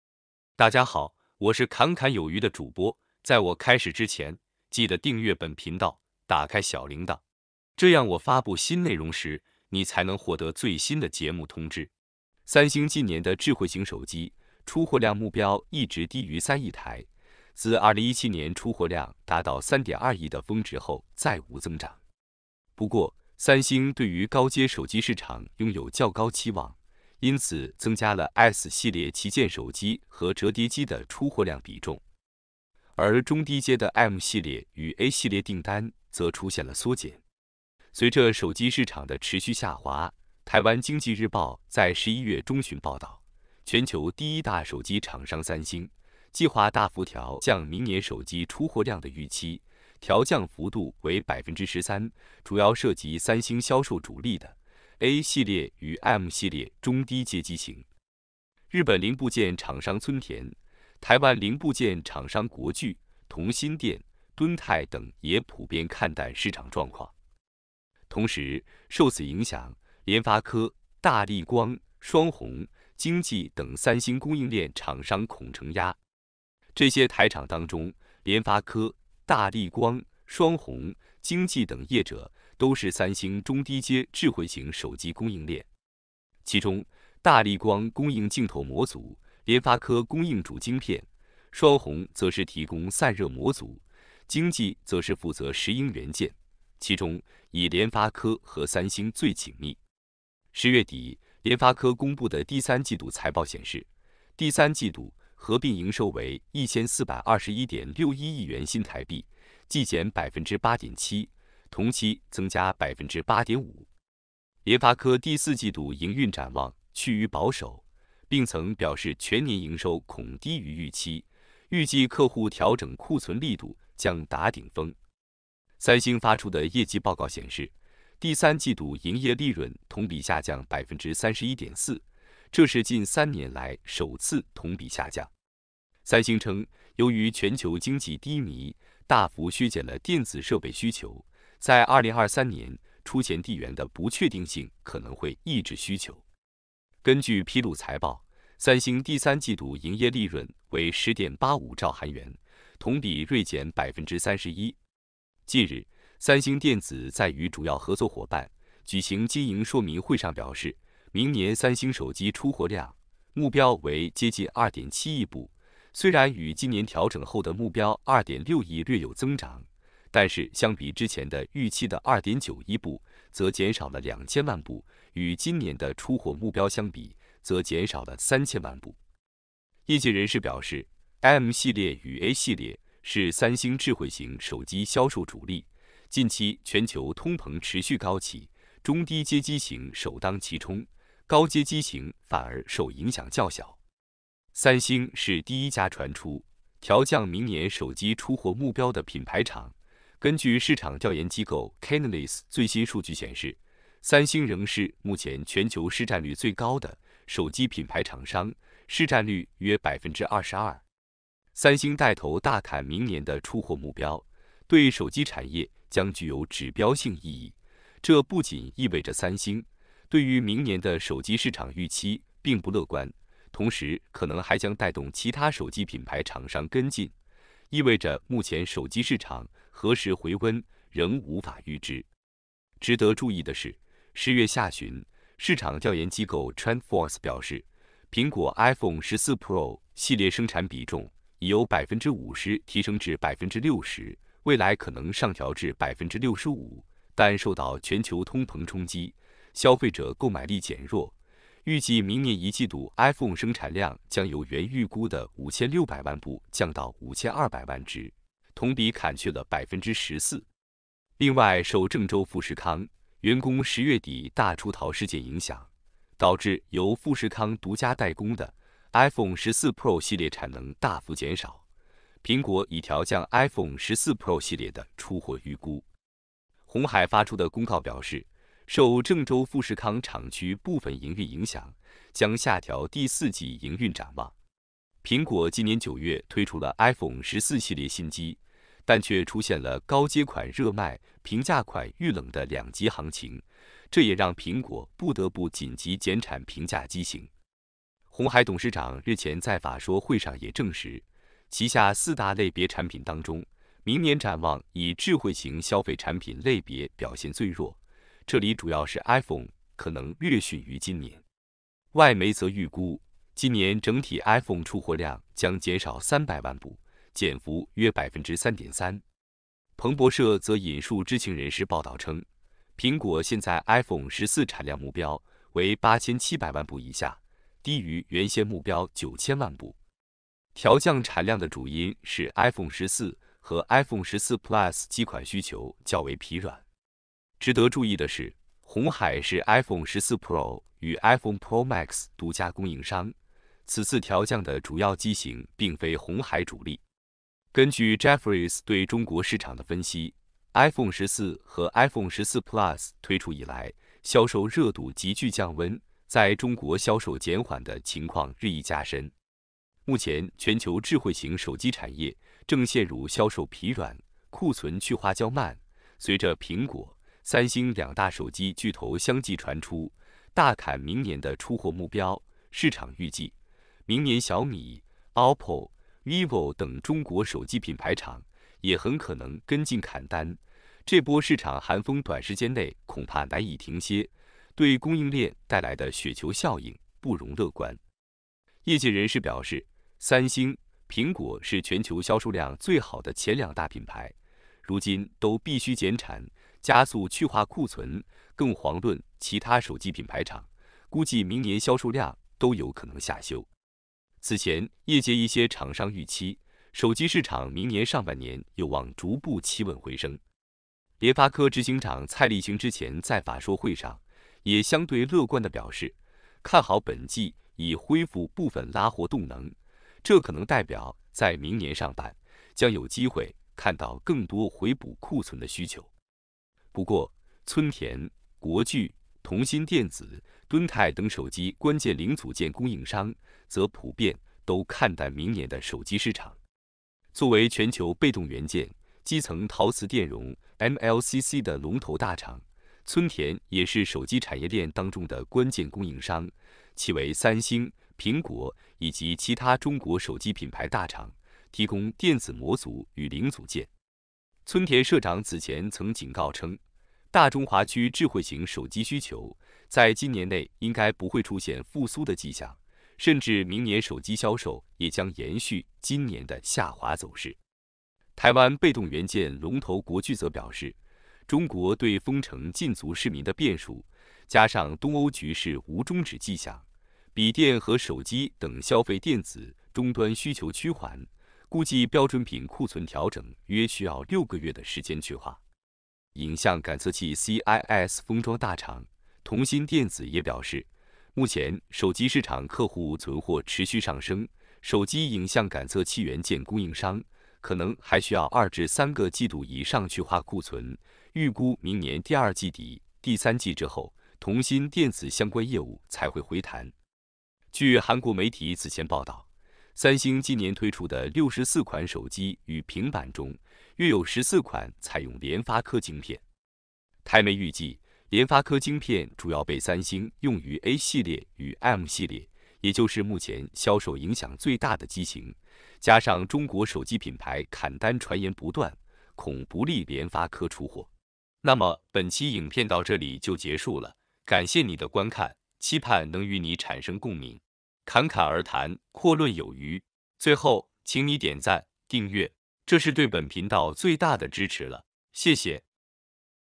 徹底傻眼！三星爆砍3000萬台手機，否則明年將堆積如山。AP晶片成燙手山芋，聯發科過於樂觀了。日本村田猛看衰市場，MLCC狂銳減。台灣關鍵零部件廠，國巨、同欣電、敦泰、大立光，模組供應鏈邁入寒冬。
全球第一大手機廠商三星計劃大幅調降明年手機出貨量的預期，調降幅度為13%。日本零部件廠商村田，台灣零部件廠商國巨、同欣電、敦泰等也普遍看淡市場狀況。

三星是第一家傳出調降明年手機出貨目標的品牌廠，根據市場調研機構Canalys最新數據顯示，三星市佔率約22%。三星帶頭大砍明年的出貨目標，對手機產業將具有指標性意義。

全球智慧型手機產業正陷入銷售疲軟、庫存去化較慢，隨著蘋果、三星兩大手機巨頭相繼傳出大砍明年的出貨目標，市場預計，明年小米、OPPO、vivo等中國手機品牌廠也很可能跟進砍單。